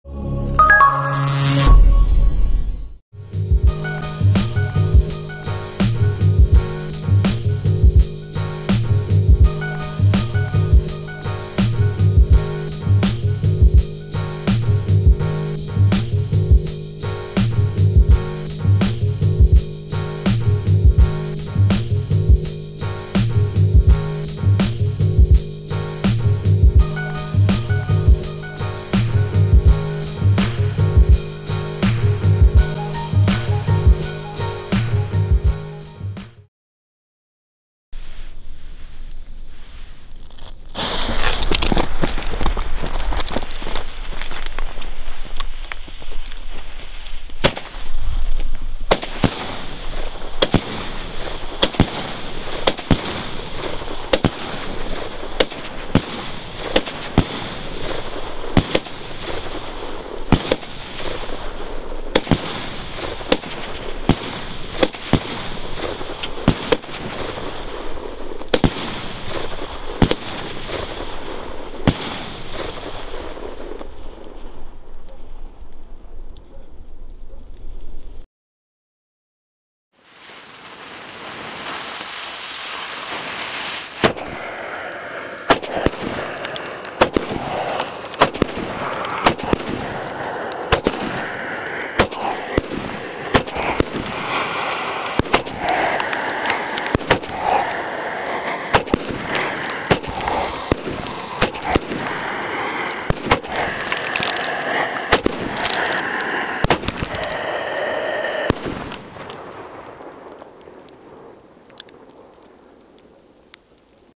Tschech. Batterie Orcs Pyro / Klasek [HD]
Tschech. Batterie Orcs Pyro von Klasek.
16 Schuss / 30mm

Pyrotechnika Kompakt 16ran / 30mm Orcs Pyro

Gefilmt und gezündet während eines Aufenthaltes in Tschechien!